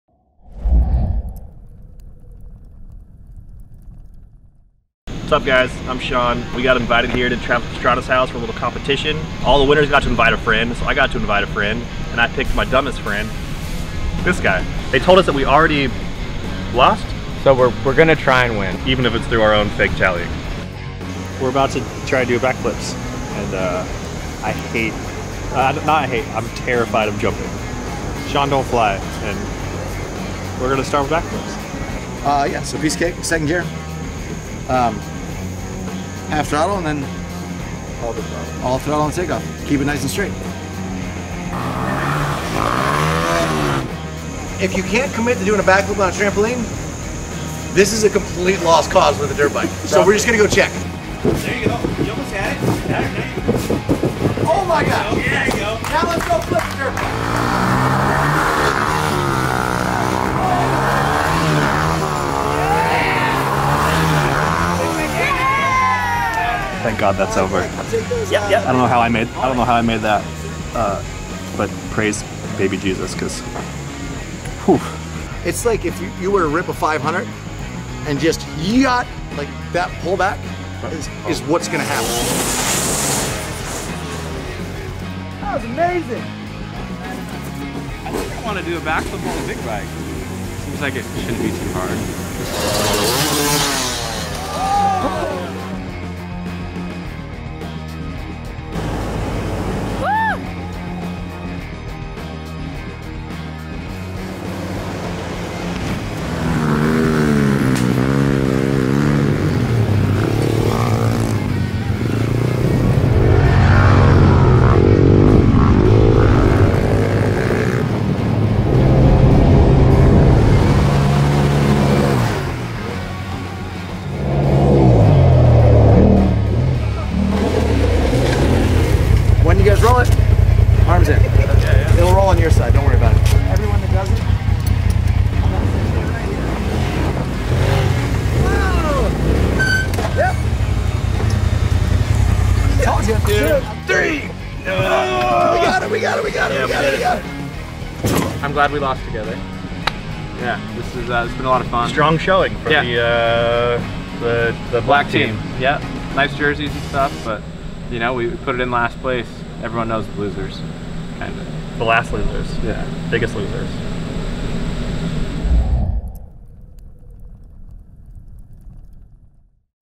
I Survived Pastranaland
Aaron Guardado from Suicide Machine Co and I head to Maryland to crash their way through Travis Pastrana's crazy compound.